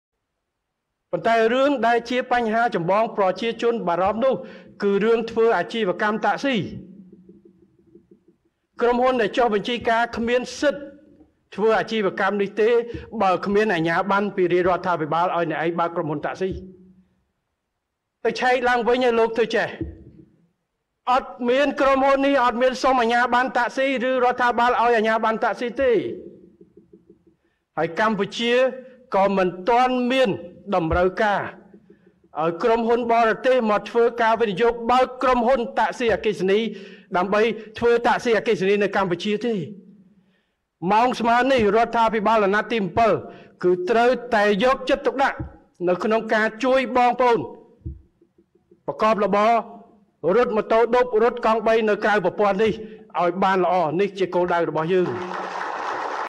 រំលឹកប្រសាសន៍សម្តេចធិបតី ហ៊ុន ម៉ាណែត ថ្លែងពីទីក្រុងតូក្យូ៖ រាជរដ្ឋាភិបាលមិនបានផ្តល់អាជ្ញាប័ណ្ណ
រំលឹកប្រសាសន៍សម្តេចធិបតី ហ៊ុន ម៉ាណែត ថ្លែងពីទីក្រុងតូក្យូ៖ រាជរដ្ឋាភិបាលមិនបានផ្តល់អាជ្ញាប័ណ្ណ ឲ្យក្រុមហ៊ុនតាក់ស៊ីអគ្គិសនី វៀតណាម បើករកស៉ីនៅកម្ពុជានោះទេ !

ប្រសាសន៍សម្តេចធិបតី ថ្លែងកាលពីខែធ្នូ ឆ្នាំ២០២៣ !